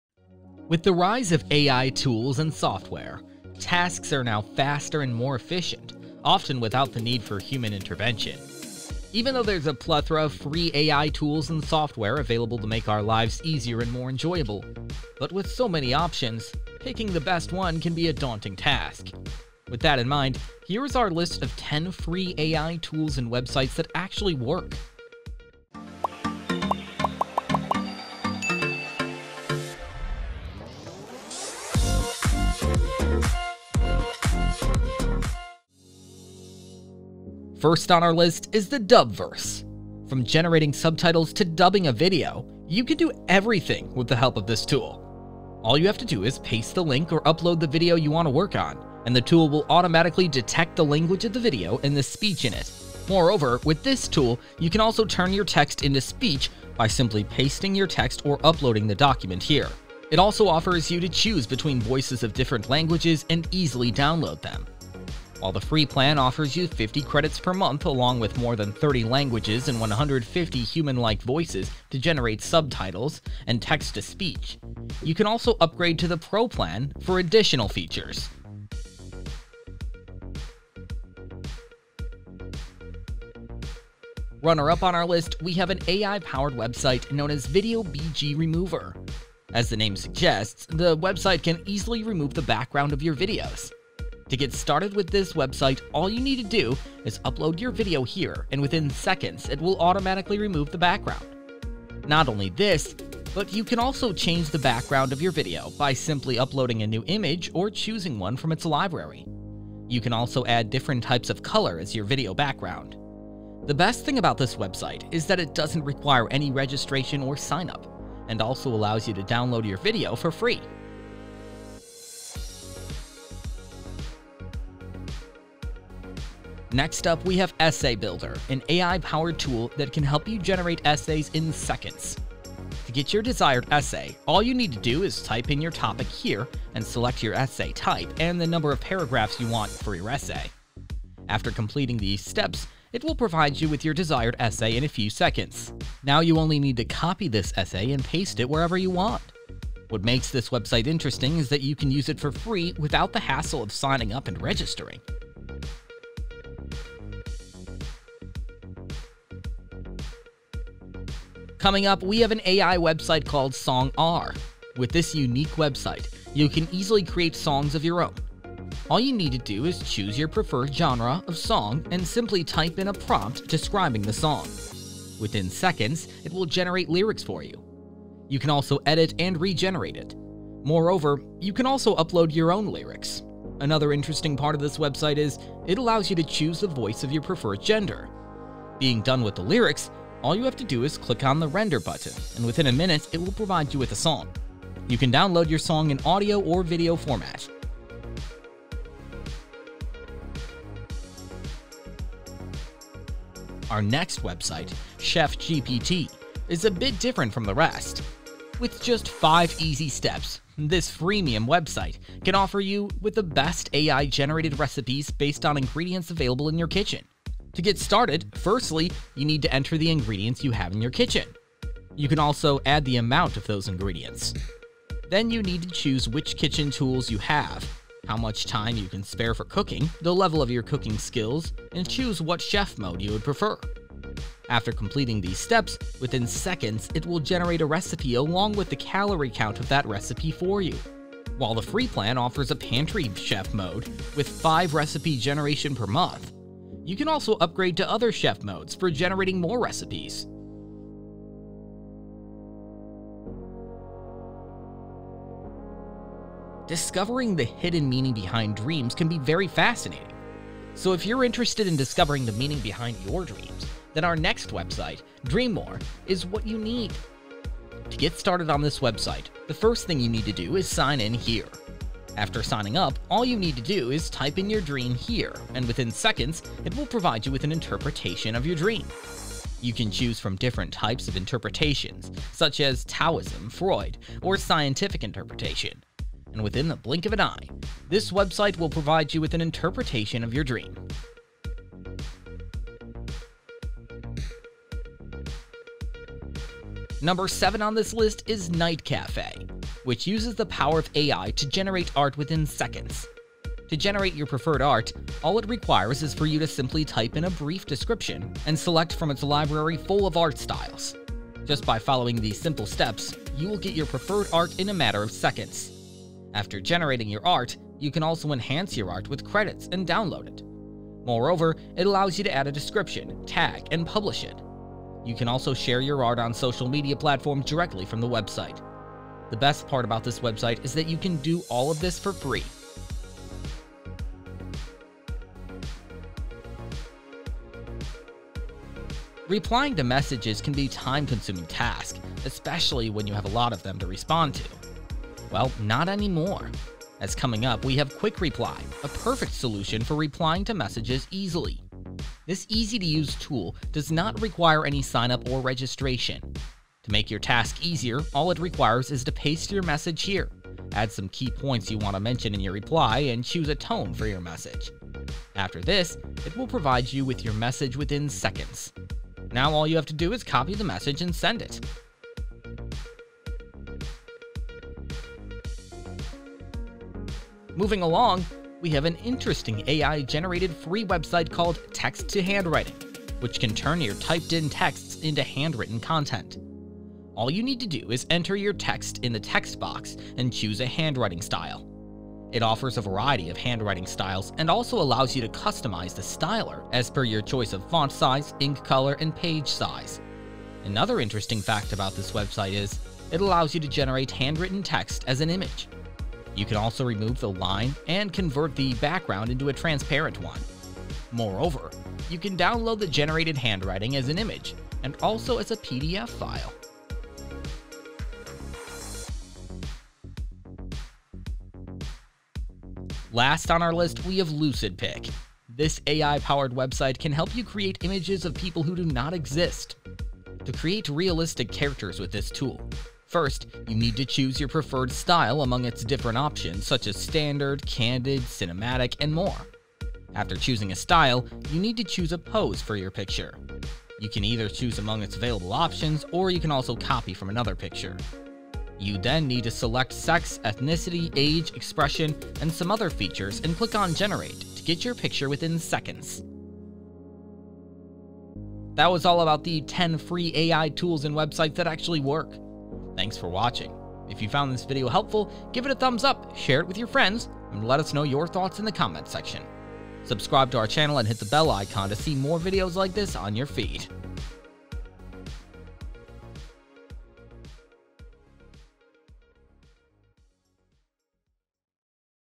10 Free AI Tools & Websites That Actually Work ▶10
With the rise of AI tools and software, tasks are now faster and more efficient, often without the need for human intervention. Even though there's a plethora of free AI tools and software available to make our lives easier and more enjoyable, but with so many options, picking the best one can be a daunting task. With that in mind, here is our list of 10 Free AI Tools & Websites That Actually Work.

10 Free AI Tools & Websites That Actually Work ▶10

Dubverse

Video BG Remover

Essay builder

SongR

ChefGPT

Dreamore

Night Cafe

Quick Reply

Text to handwriting

Lucid pic
Intro and Animation Credit:
10Studio: